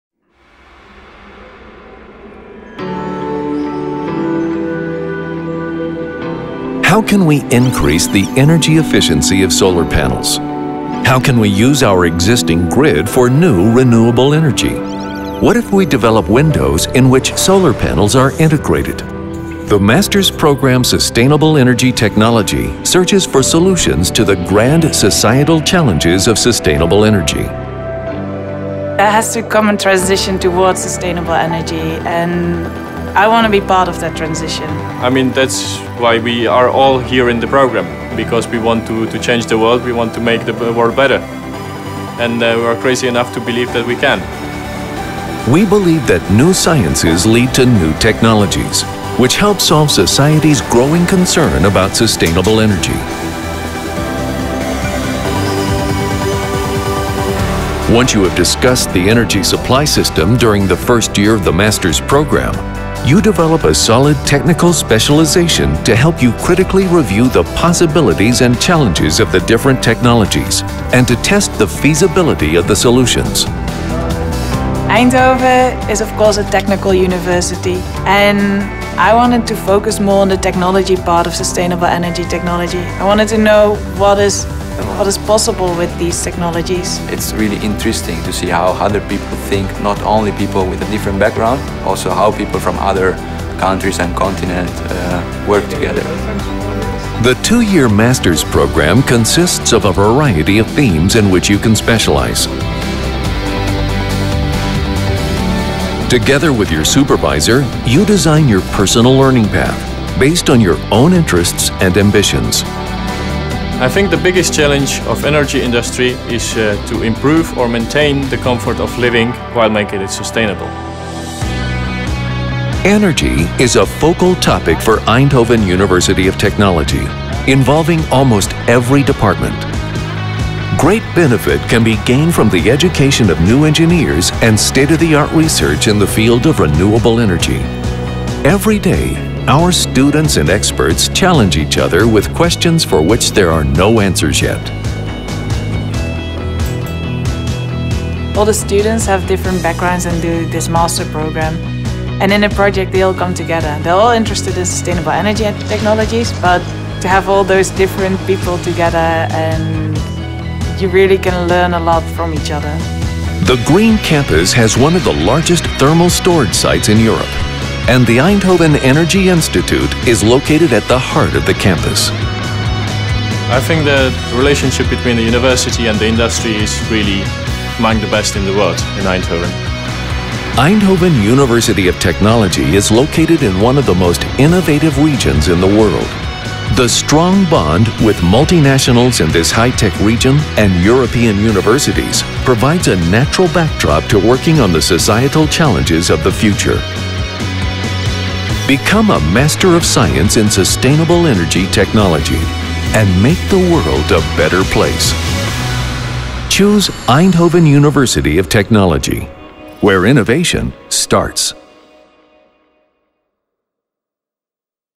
TU/e (Technische Universiteit Eindhoven) - Graduate Program Sustainable Energy Technology (SET)
As a student of the Graduate Program Sustainable Energy Technology at Eindhoven University of Technology you will be searching for solutions to the grand societal challenges of sustainable energy.

In opdracht van de TU/e geproduceerd door Veldkamp Produkties.

Altijd op de hoogte blijven van nieuwe films in ons portfolio?